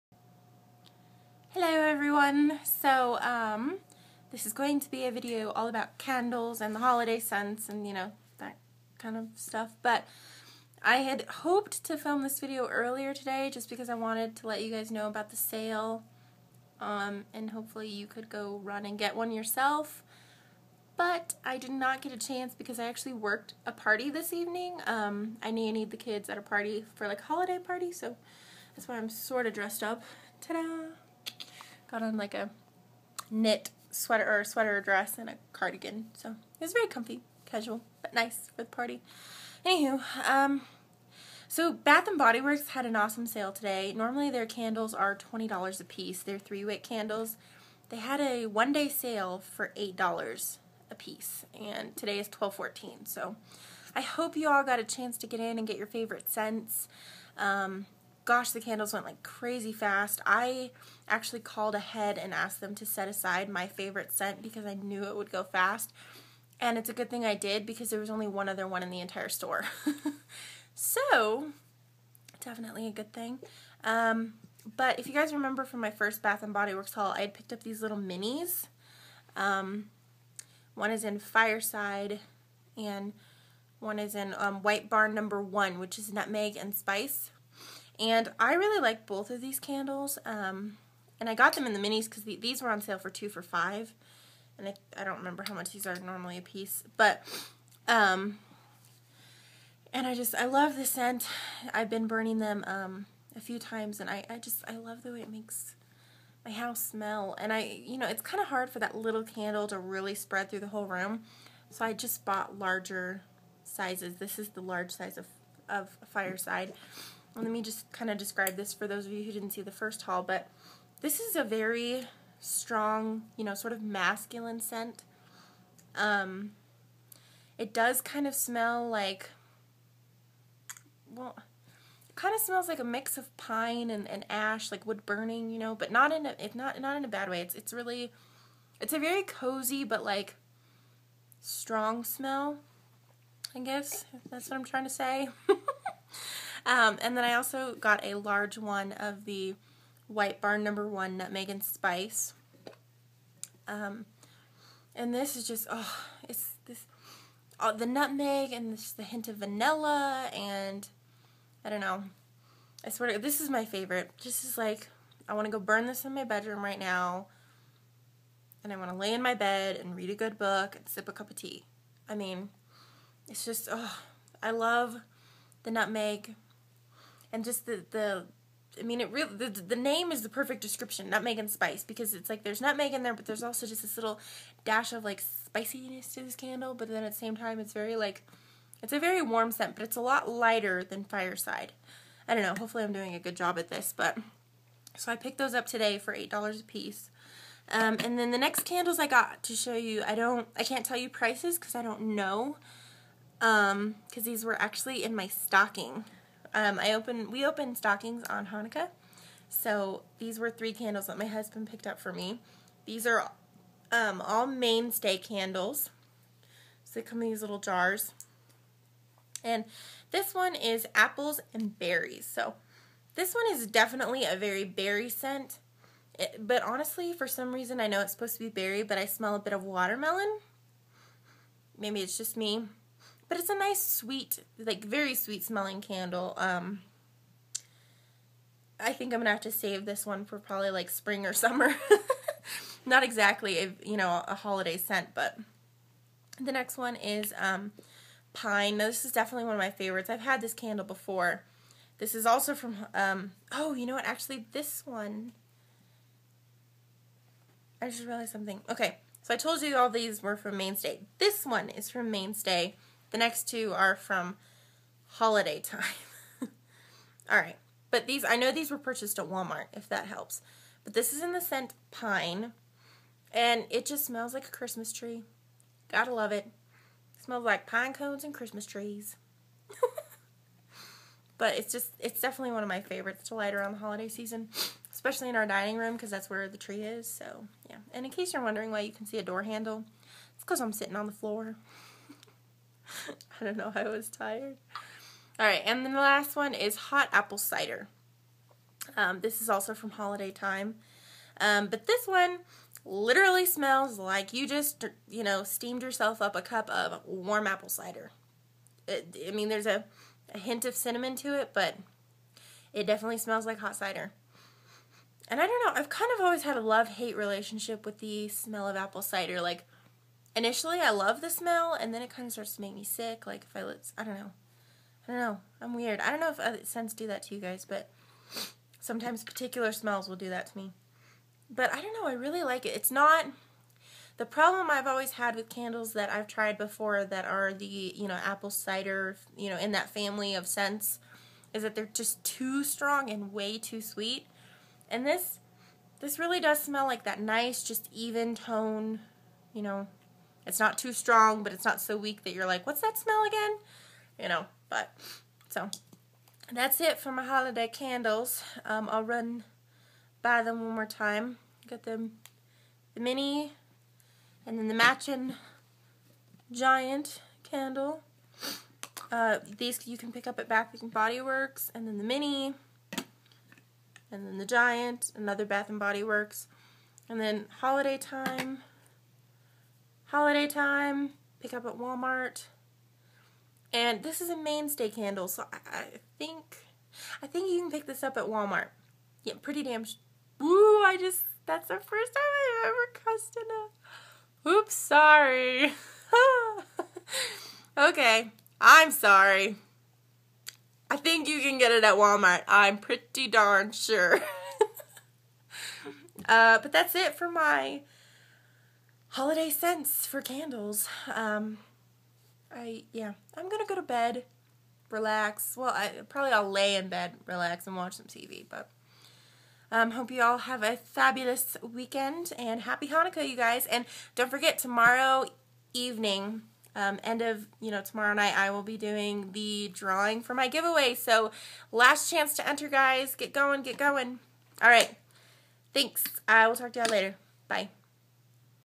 Holiday Candles!
Just wanted to mention my favorite holiday candles and some favorite scents I picked up on sale today.